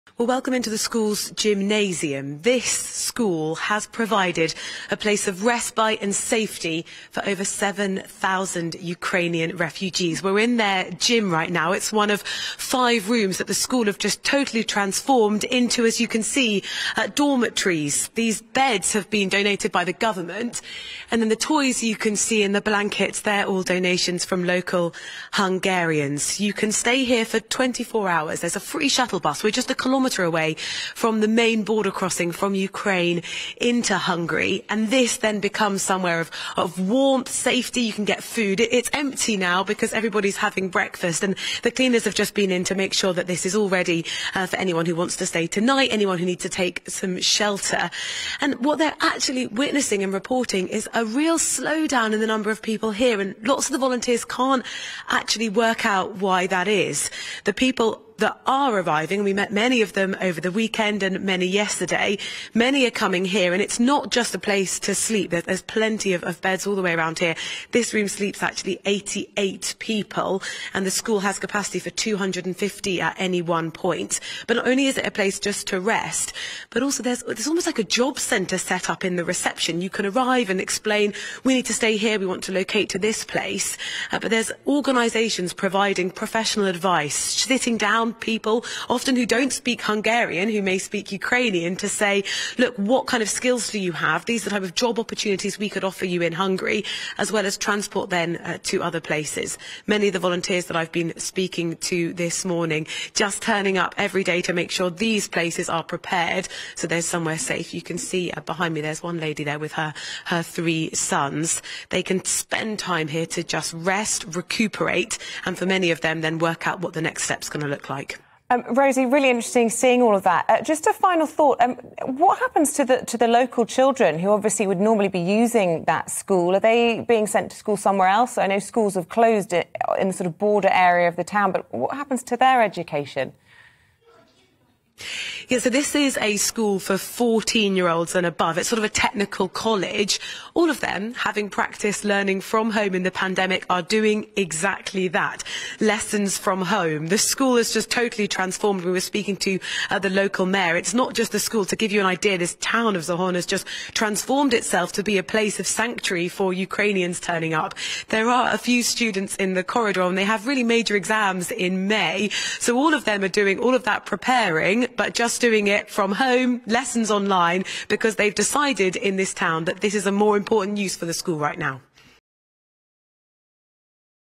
Refugee crisis: ‘This school has provided a place of respite and safety for over 7000 Ukrainians'
‘This school has provided a place of respite and safety for over 7000 Ukrainians'

GB News’ Rosie Wright reports from Zahony, Hungary where a school has been converted into a help point for fleeing refugees.